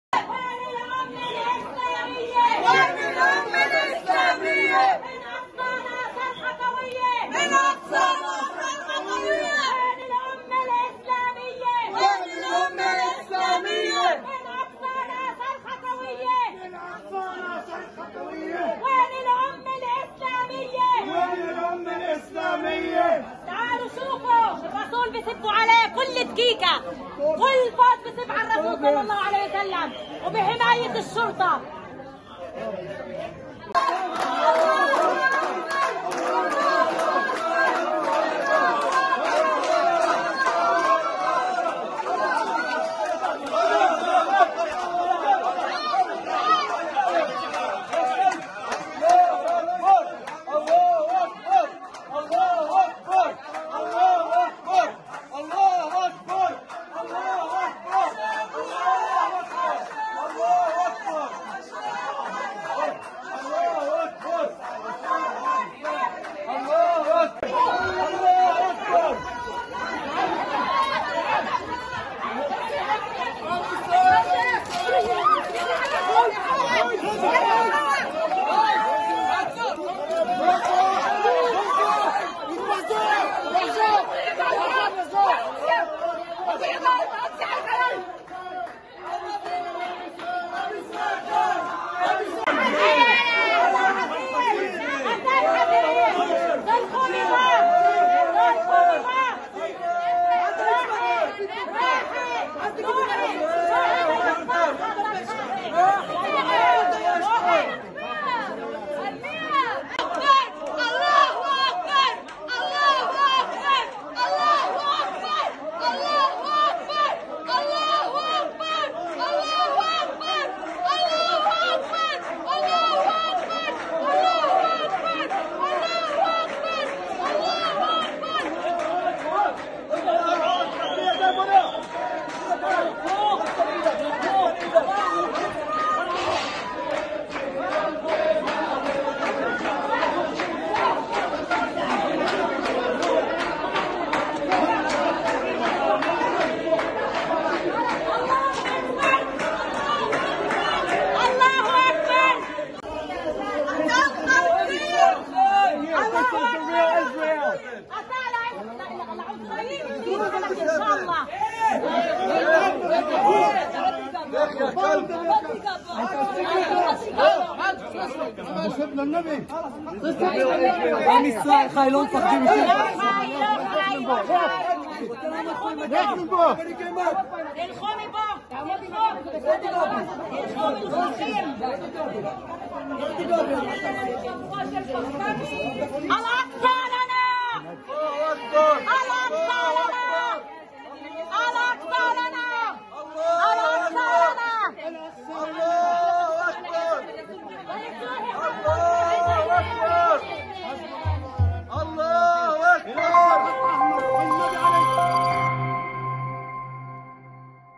اعتداء المستـ ـوطنين وشرطة الاحـ ـتلال على المر!بطين في منطقة باب السلسلة أحد أبواب المسجد الأقصى
شاهد كيف اعتدى المستـ ـوطنون وشرطة الاحـ ـتلال على المر!بطين في منطقة باب السلسلة أحد أبواب المسجد الأقصى المبارك.